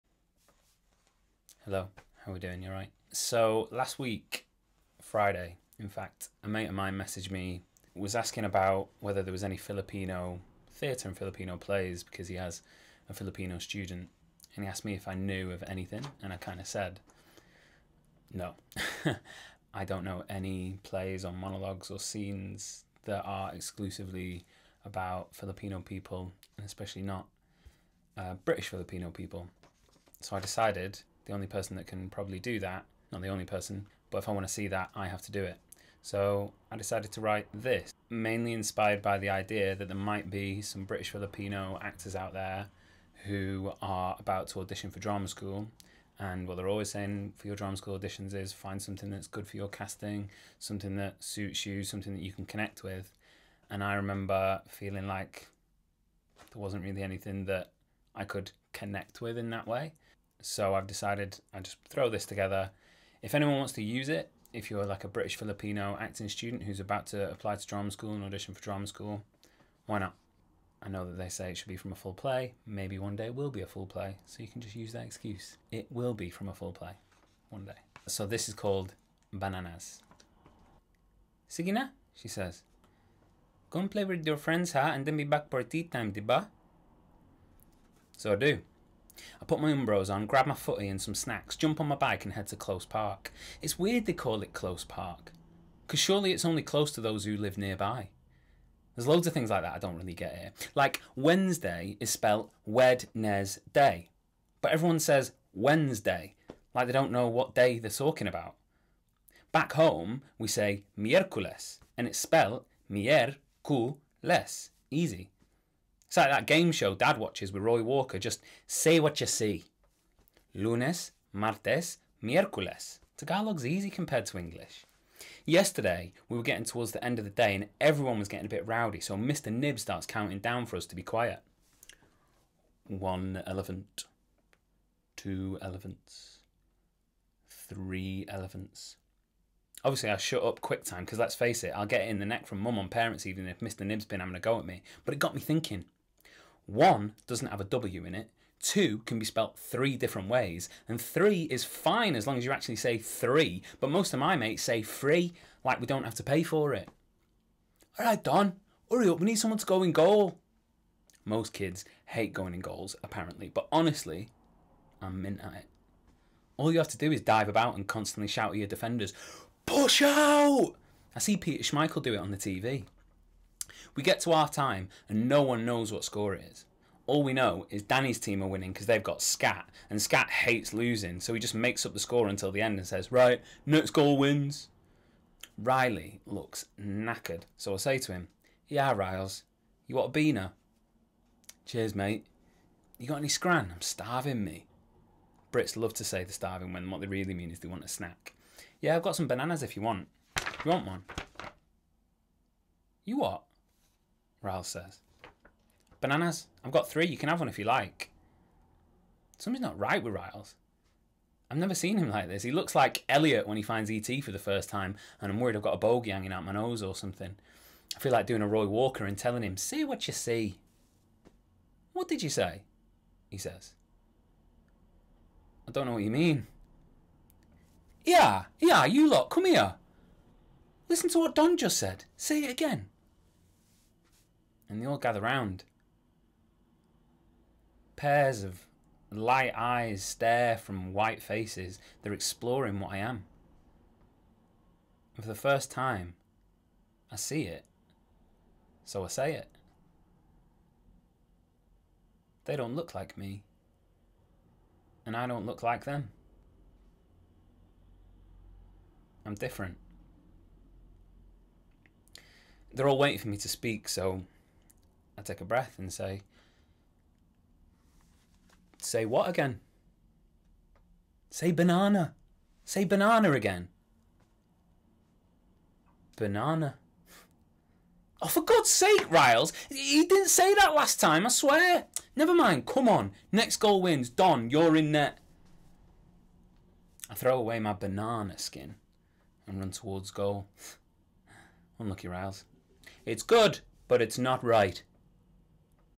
Bananas by James Cooney
A monologue for British Filipino actors. Bananas explores what it's like to be a Filipino growing up in 90s England.